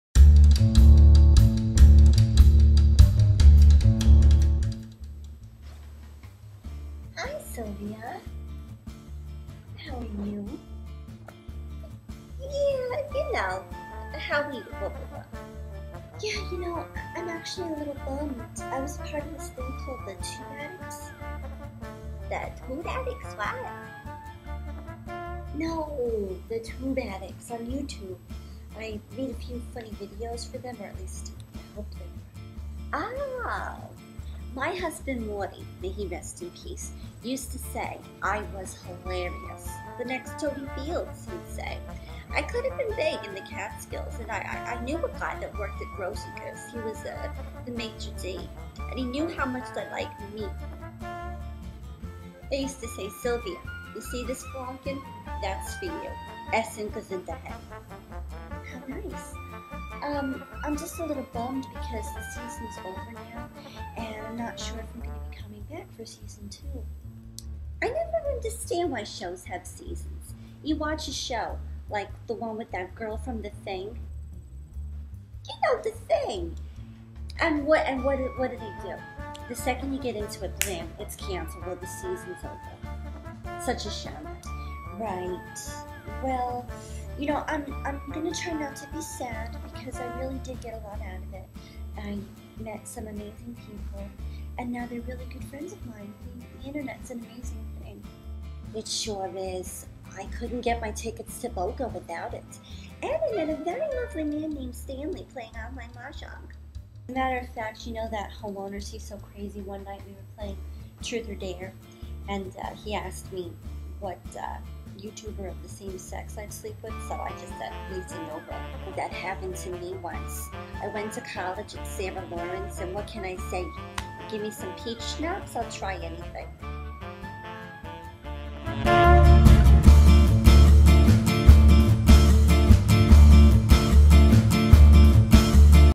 Saturdays With Sylvia
SEE YOU IN SEASON 2! TA-TA FOR SATURDAYS!

I had a little chat with my Aunt Sylvia. I don't know if you'll notice this, but she's kinda lacking in conversational skills. I hope you've all enjoyed my Saturdays on TubeAddicts. It's been a pleasure to perform for you! Love you all!

SEE YOU IN SEASON 2!